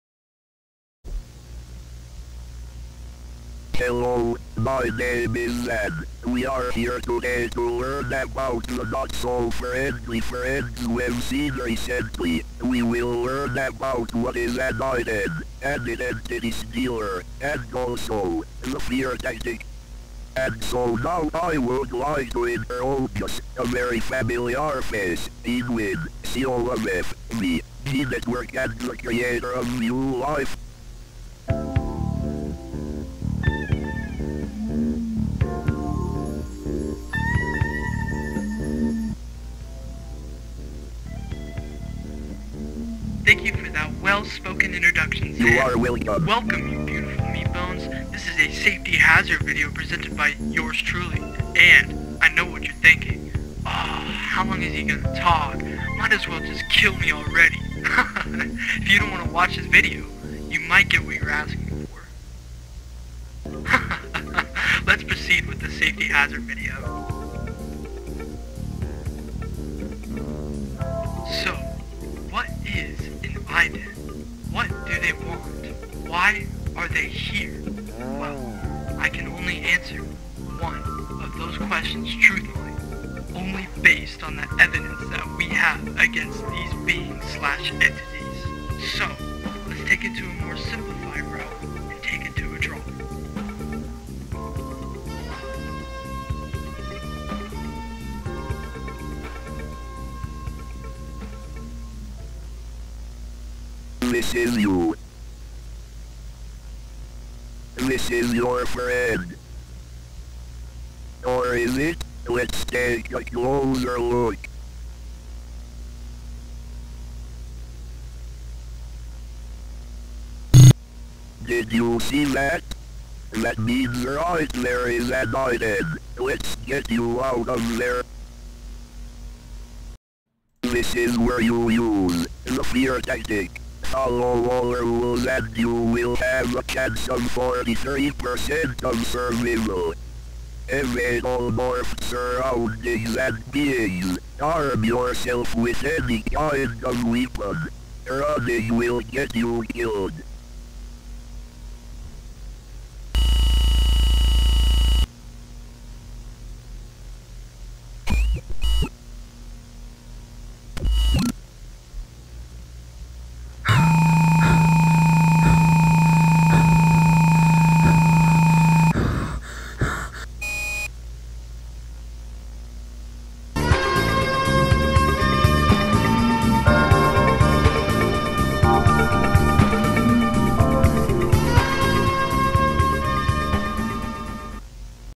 A VIEW LIFE SAFETY HAZARD VIDEO (SKIN OF LIMINALITY)
A View Life safety hazard video. SKIN OF LIMINALITY is a web series coming soon
if you enjoy the analog horror that's where I'm going for.